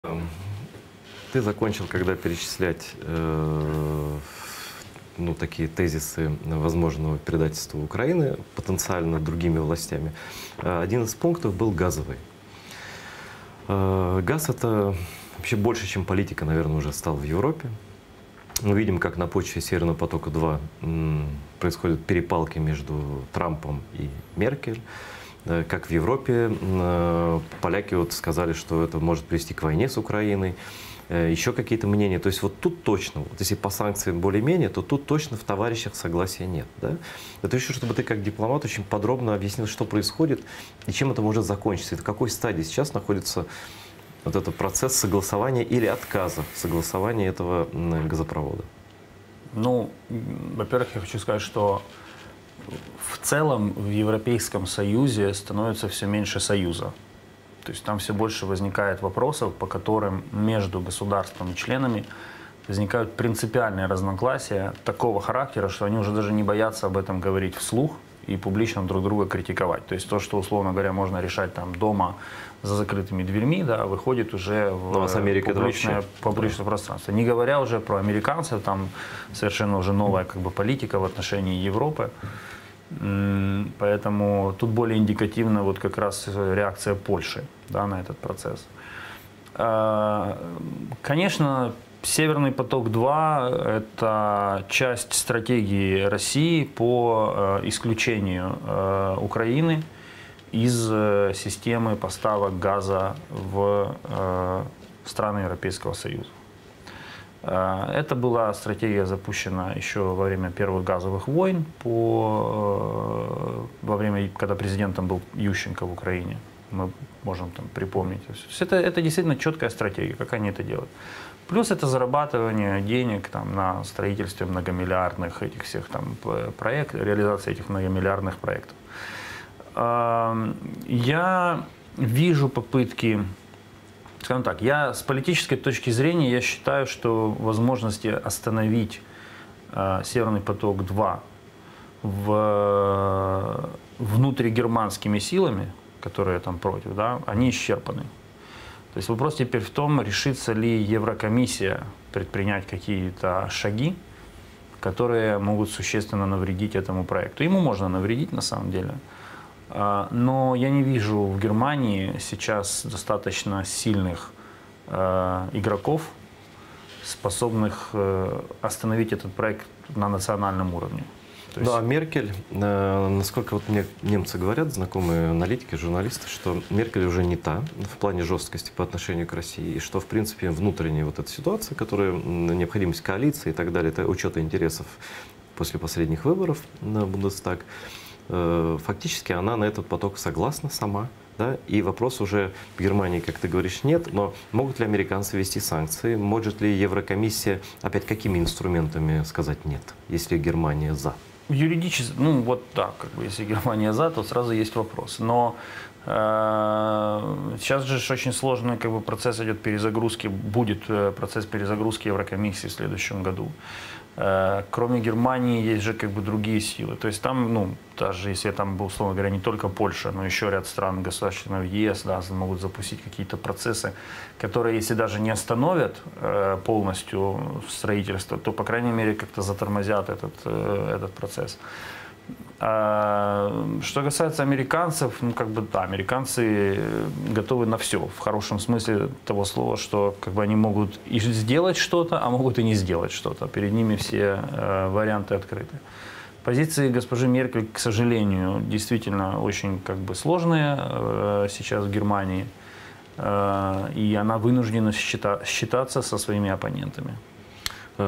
Кулеба: В Германии нет сильных игроков, способных остановить строительство «Северного потока-2»
PRIME:МУЖДАБАЄВ
Гость: Дмитрий Кулеба — постоянный представитель Украины при Совете Европы.